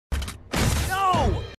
My Reaction To Undergrads, Yin Yang Yo! & Disney Packages From Planet X Ending In 2014 Got Cancelled
Undergrads, Yin Yang Yo! & Disney Packages From Planet X Got Cancelled End NOTE : I Like Lightyear 😱